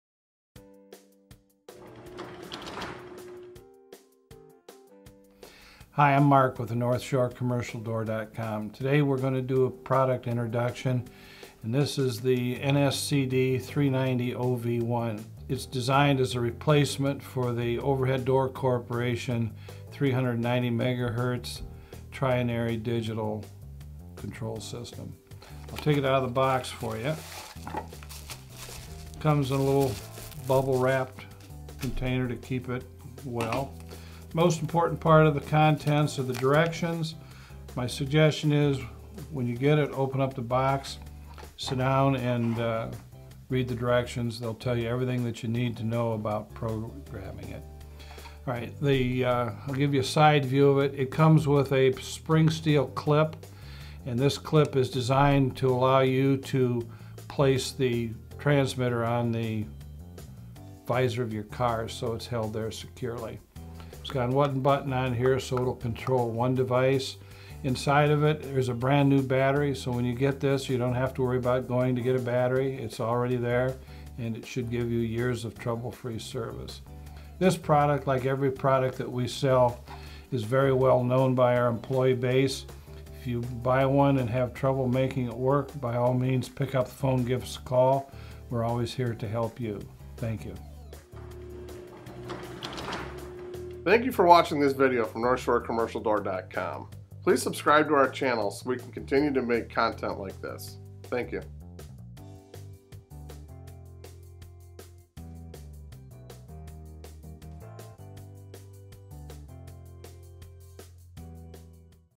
North Shore Commercial Door NSCD-390OV1 Transmitter Remote
Replace your old, defective remote with our inexpensive, compatible NSCD-390OV1 garage door/gate opener remote from North Shore Commercial Door. This remote has excellent range and reliability, crafted of premium materials. But it's a much cheaper remote because we manufacture it at our facilities, cutting out the middleman. The attached visor clip allows you to handily operate this compact 390 MHz transmitter  from inside your vehicle. When you set your code using the dipswitches, make sure the codes match on the transmitter and receiver. A long-lasting 9-volt battery is included. It's compatible with models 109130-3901, 390ODD31V, O220-390, O219-390 and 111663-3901. If you have questions, consult our expert staff.

Product Highlights:
390 MHz
Visor clip attached
9V long-life battery
9 dipswitches
Excellent range
In-house brand = lower price
Compatible with models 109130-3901, 390ODD31V, O220-390, O219-390, and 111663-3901

NorthShoreCommercialDoor.com Production